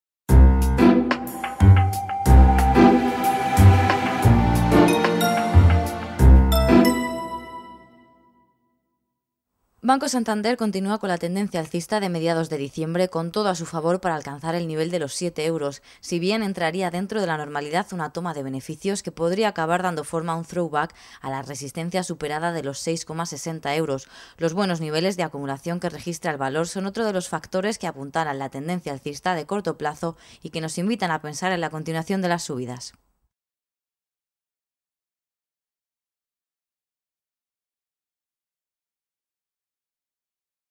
Análisis técnico de Banco Santander
Entraría dentro de la normalidad una toma de beneficios que podría acabar dando forma a un throw back a la resistencia superada de los 6,60 euros.